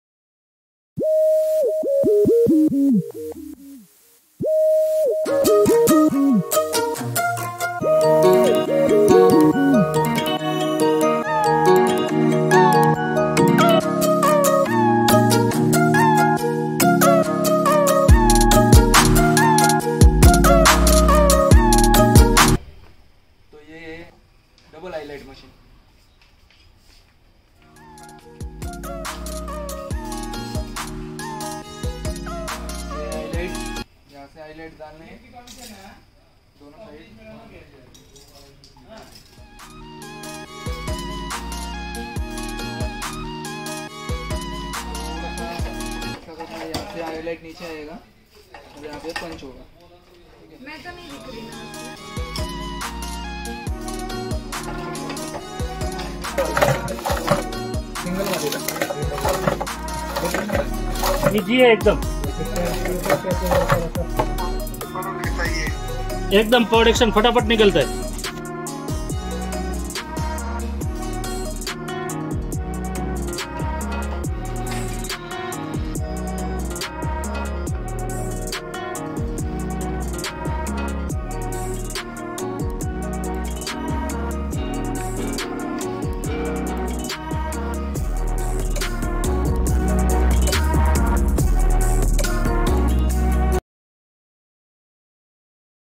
DOUBLE EYELET MACHINE | Shiv Enterprises pune | 9372419599
DOUBLE EYELET MACHINE

Products information:
Eyeleting Diameter : 4mm
Binding Thickness : ≤ 3mm
Vertical Working Travel : 60mm - 165mm
Hold Setting Range : 80 - 130mm
Vessel Volume (Container Capacity of Eyelet) : 1200 Grain
Gross Weight : 28 Kgs
Overall Size : 44*42*39.5cm